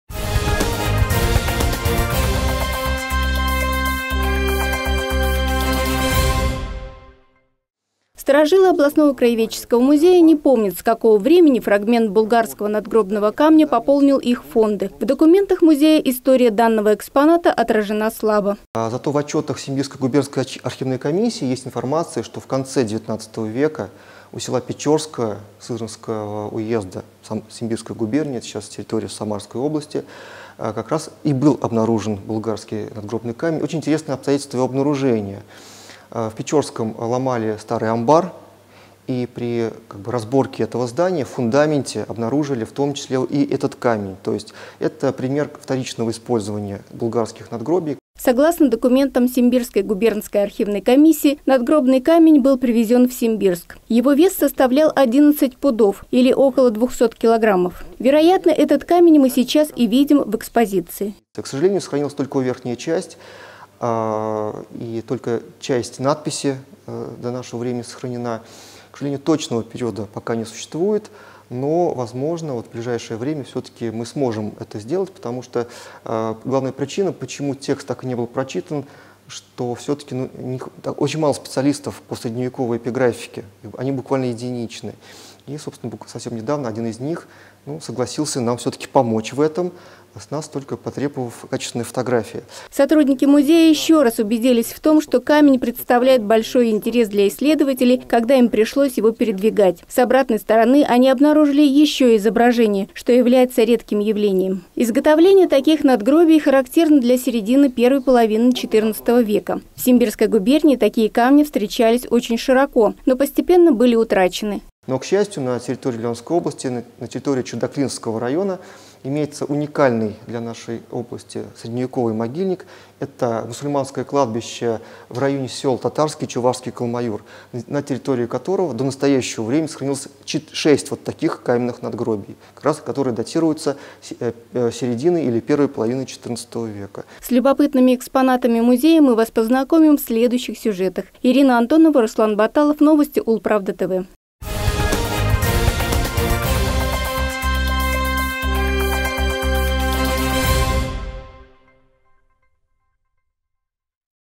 История булгарского надгробного камня в виртуальной экскурсии по краеведческому музею
Продолжаем виртуальные экскурсии по музеям. Одним из наиболее примечательных экспонатов коллекции Ульяновского областного краеведческого музея является фрагмент булгарского надгробного камня, изготовленного из поволжского песчаника.